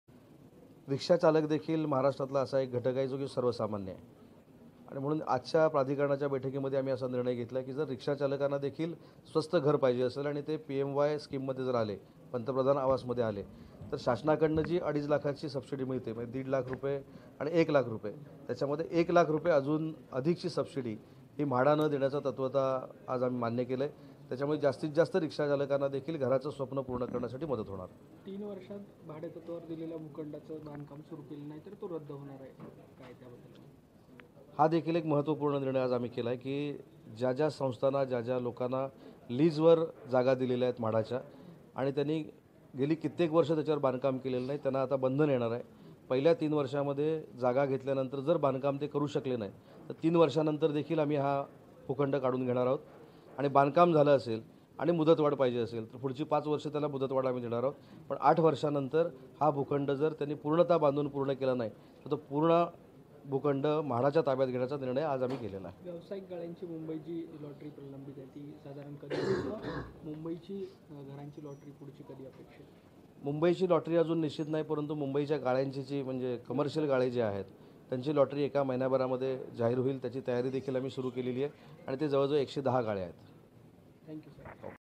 म्हाडा देणार रिक्षाचालकांना एक लाखाचे अतिरिक्त अनुदान
मुंबई - रिक्षाचालकांना स्वतःच्या हक्काचे घर मिळावे, यासाठी म्हाडाने नवा निर्णय घेतला आहे. ग्रामीण शहरी भागातल्या रिक्षा चालक-मालकांना एक लाखाचे अतिरिक्त अनुदान म्हाडाकडून देण्यात येणार आहे.